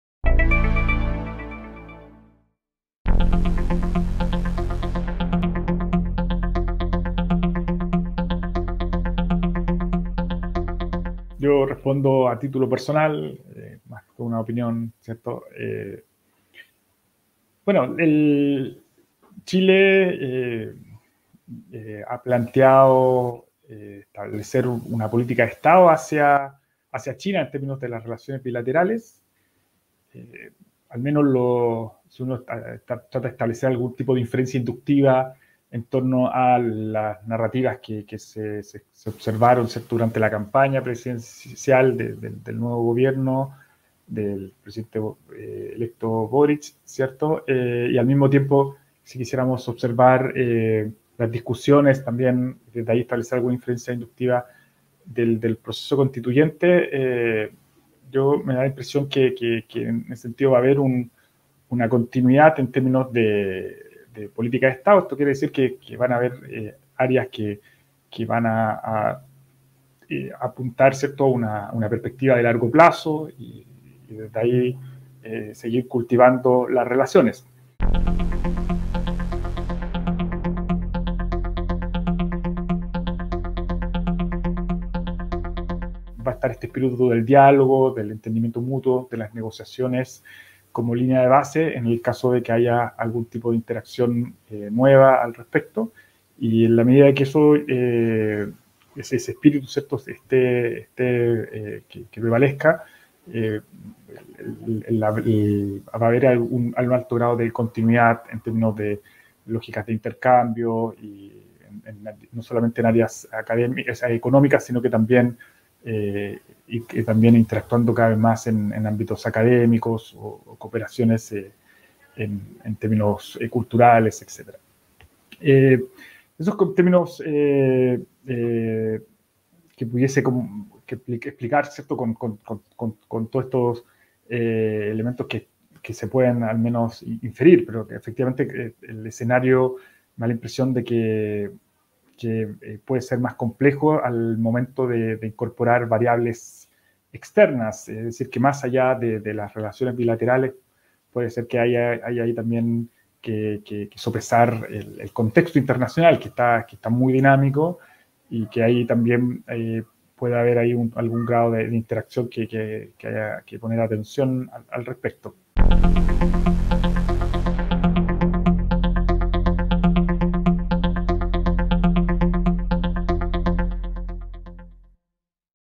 Andrés Bórquez analiza la relación Chile China en un contexto de cambios globales
Profesor asistente y coordinador de Programa de Estudios Chinos del Instituto de Estudios Internacionales de la Universidad de Chile, analizó el futuro de la relación Chile-China tomando en consideración el contexto de cambios, tanto a nivel nacional como global. Más detalles de esta entrevista en la nota publicada por el Programa Asia Pacífico de la BCN